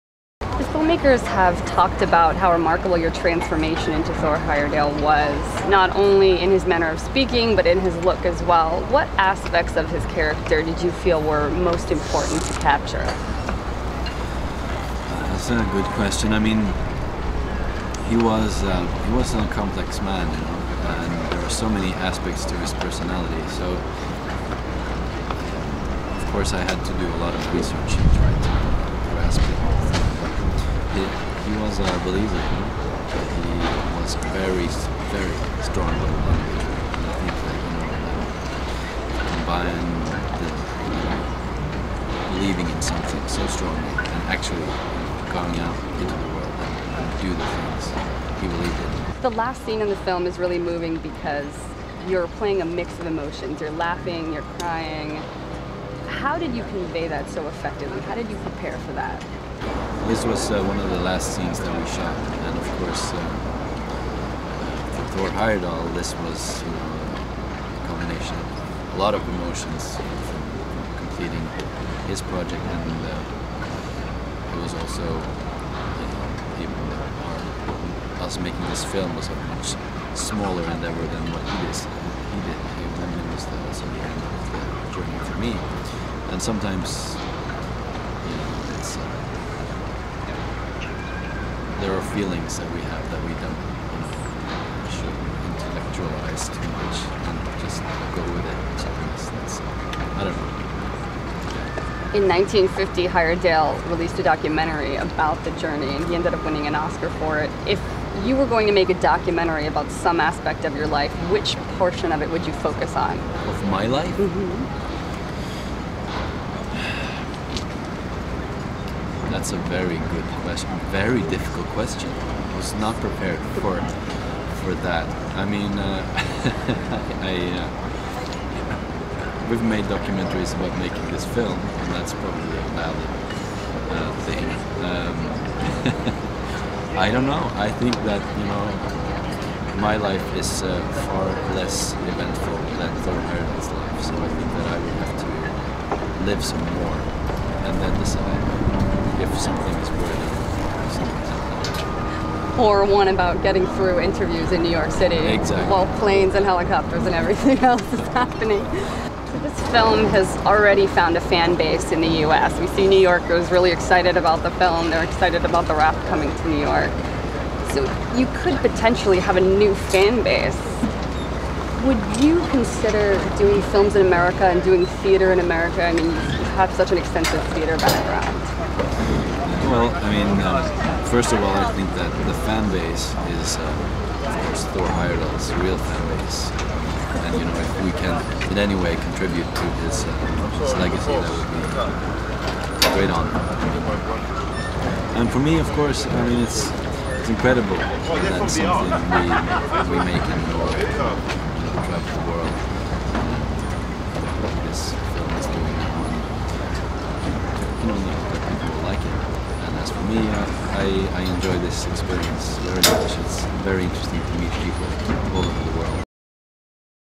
Pål Sverre Hagen "Kon Tiki" Interview
Justine Browning chats with actor Pål Sverre Hagen about "Kon Tiki."